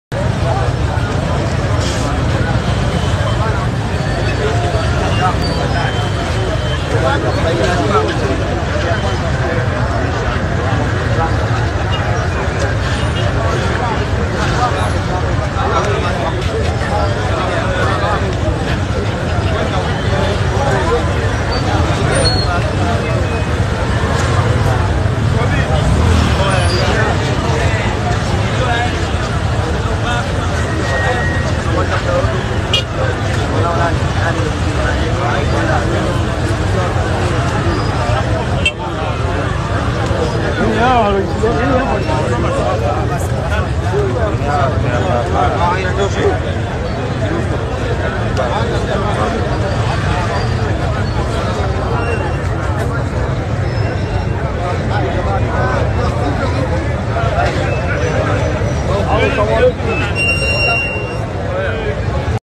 MAELFU YA WAJITOKEZA KUMZIKA ALLY SONSO..
Burian Ally Sonso Mwili wa Marehemu wapumzishwa katika nyumba yake ya milele. Pumuzika kwa Amani yanga_arena yanga (1).mp4